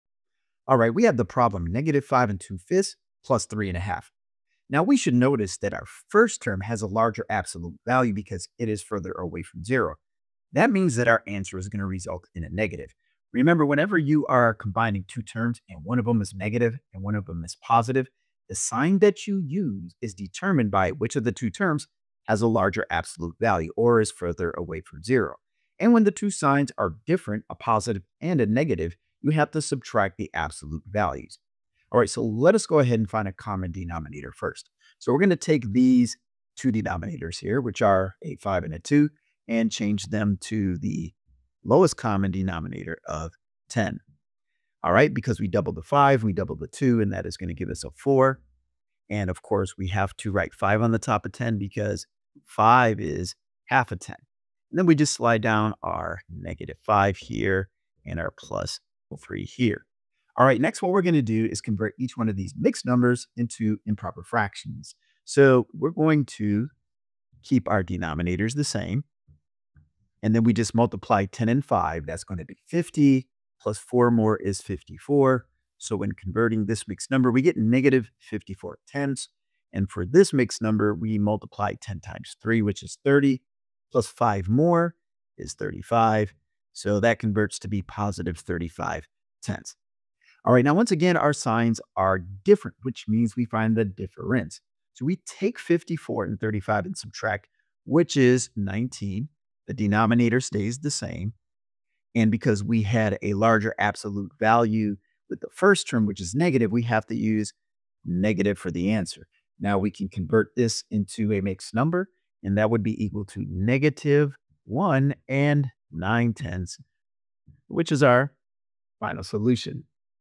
How to Add and Subtract Mixed Numbers with Negatives
In this lesson, we'll walk through how to subtract and add negative mixed numbers. You'll learn vital skills like renaming mixed numbers, converting them to improper fractions, and finding a common denominator to accurately perform operations. We’ll also explain the rules of adding a negative, how absolute value plays a role, and how to simplify your answers.

What You'll Learn:

Renaming mixed numbers for clarity

Converting mixed numbers → improper fractions

Finding common denominators for mixed fractions

Applying the rules of adding and subtracting negatives

Using absolute value to make sense of negative results